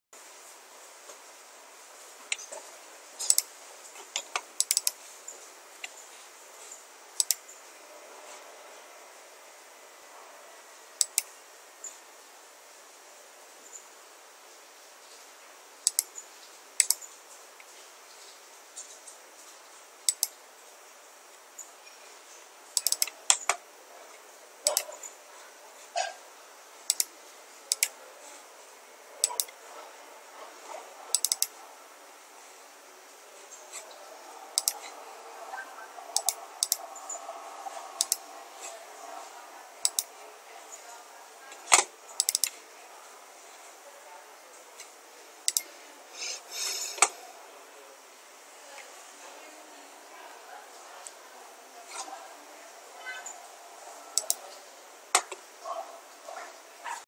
What is denoise of microscope imaging
Through the denoise function of microscope imaging software, the user can eliminate the most noise on the image to get a good performance micro image. For more information on the software and camera, welcome